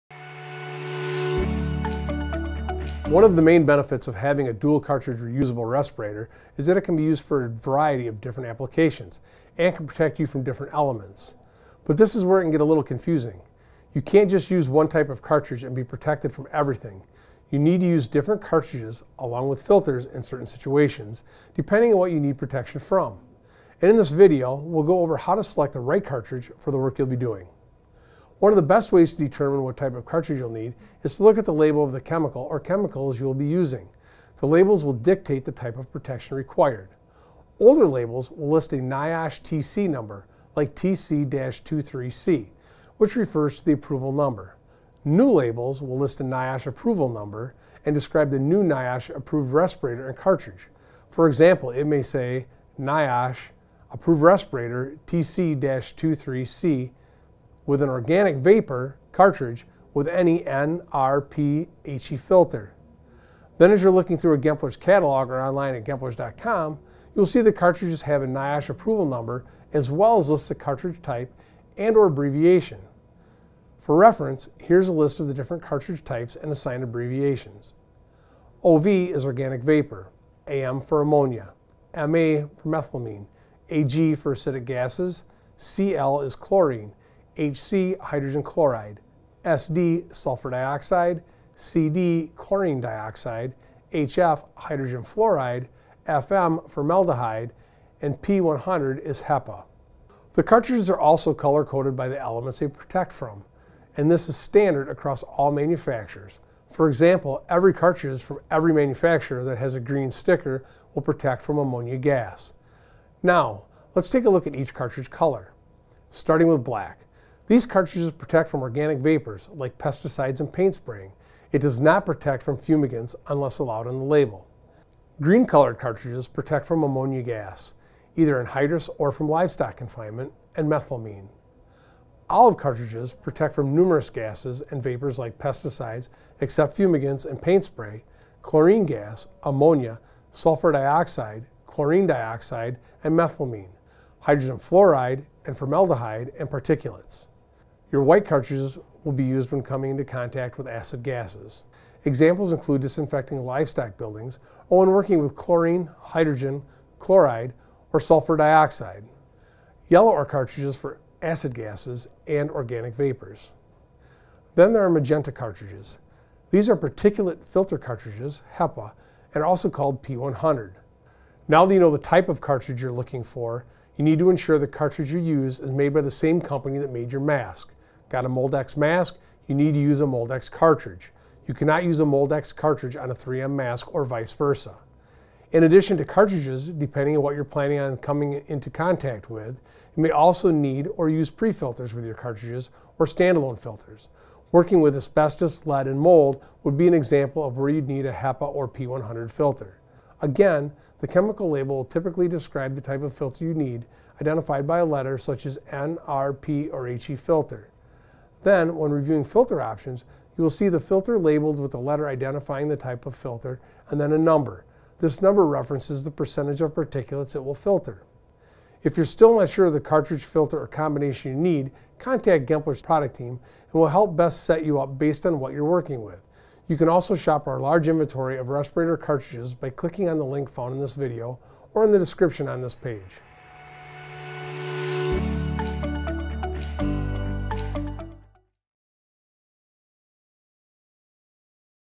Selecting the Right Reusable Respirator Cartridge Filter
Make sure to select the right cartridge filter for your reusable respirator to get the protection you need for what you're working with.

One of the main benefits of having a dual cartridge reusable respirator is that it can be used for a variety different applications and can protect you from different elements. But this is where it can get a little confusing. You can't just use one type of cartridge and be protected from everything. You need to use different cartridges, along with filters in certain situations, depending on what you need protection from and in this video we'll go over how to select the right cartridge for the work you'll be doing.

One of the best ways to determine what type of cartridge you'll need is to look at the label of the chemical or chemicals you will be using. The labels will dictate the type of protection required. Older labels will list a NIOSH TC number like TC-23C, which refers to the approval number. New labels will list a NIOSH approval Number and describe the new NIOSH-approved respirator and cartridge. For example, it may say: NIOSH-approved respirator TC-23C with an organic vapor cartridge with any N, R, P, HE filter.

Then, as you're looking through a Gemplers catalog or online at gemplers.com, you will see the cartridges have a NIOSH Approval number as well as list the cartridge type and/or abbreviation. For reference, here is a list of the different cartridge types and assigned abbreviation.

OV is Organic Vapor
AM for Ammonia
MA for Methylamine
AG for Acidic gasses
CL is Chlorine
HC Hydrogen Chloride
SD Sulfur Dioxide
CD Chlorine Dioxide
HF Hydrogen Fluoride
FM Formaldehyde, and
P100 is HEPA

The cartridges are also color coded by the elements they protect from and this is standard across all manufacturers. For example, every cartridge, from every manufacturer, that has a green sticker, will protect from Ammonia gas.

Now, let's take a look each cartridge color.

Starting with black, these cartridges protect from organic vapors: like pesticides and paint spraying. It does not protect from fumigants, unless allowed on the label.

Green colored cartridges protect from Ammonia gas, either anhydrous or from livestock confinement, and methylamine.

Olive cartridges protect from numerous gases and vapors like pesticides (except fumigants and paint spray); chlorine gas; ammonia; Sulfur dioxide, chlorine dioxide and methylamine; hydrogen fluoride and formaldehyde; and particulates.

Your white cartridges will be used when coming into contact with acid gases. Examples include disinfecting livestock buildings, or when working with chlorine, hydrogen, chloride or Sulfur dioxide.

Yellow are cartridges for acid gases and organic vapors.

Then there are the magenta cartridges. These are particulate filter cartridges (HEPA) - and are also called P100.

Now that you know the type of cartridge you're looking for, you need to ensure the cartridge you use is made by the same company that made your mask.  Got a Moldex mask? You need to use Moldex cartridge. You cannot use Moldex cartridge on a 3M mask or vice versa.

In addition to cartridges, depending on what you're planning on coming into contact with, you may also need or use pre-filters with your cartridges or stand-alone filters. Working with asbestos, lead and mold would be an example of where you would need a HEPA or P100 filter.

Again, the chemical label will typically describe the type of filter you need, identified by a letter such as N, R, P or HE filter.

Then, when reviewing filter options, you will see the filter labeled with the letter identifying the type of filter and then a number. This number is referencing the percentage of particulates it will filter.

If you're still not sure the cartridge, filter or combination you need, contact Gemplers Customer Support at and we'll help best set you up based on what you're working with.

You can also shop our large inventory of respirator cartridges and find out more about respirator types, use and care